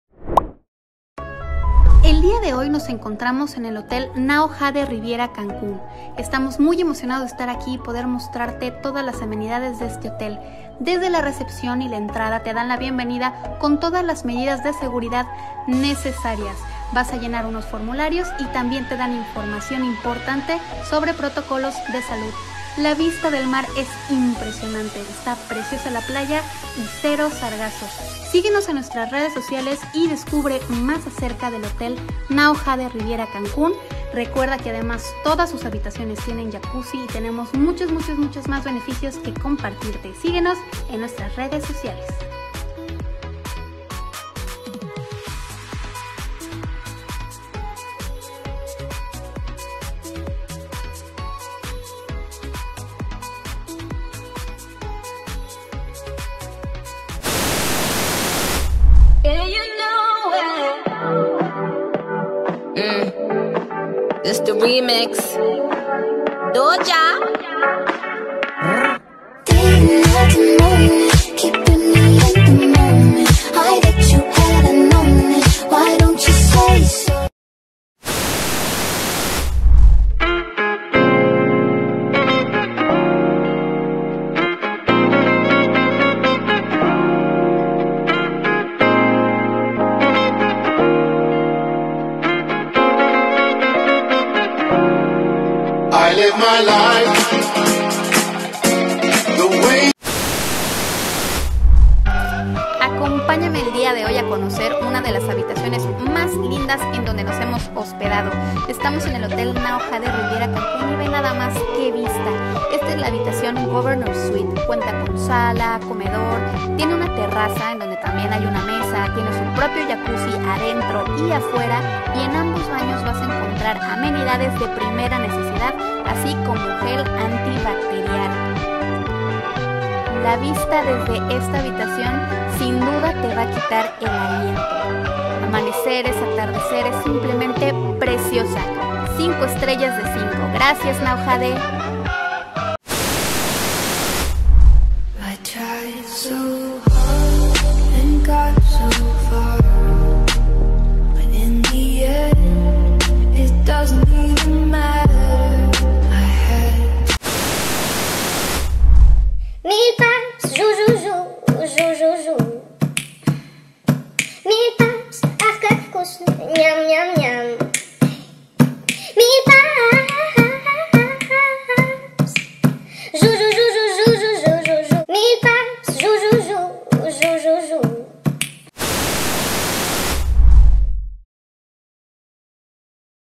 Un día en Now Jade Riviera Cancún | OCTUBRE 2020 | Compilación TikToks PriceTravel.com
Descubre con nosotros Now Jade Riviera Cancún, hermoso hotel con plan todo incluido de lujo y con Alto Estándar de Limpieza. Acompáñanos a ver qué encontramos, anota información importante para tu viaje y reserva tu viaje con nosotros.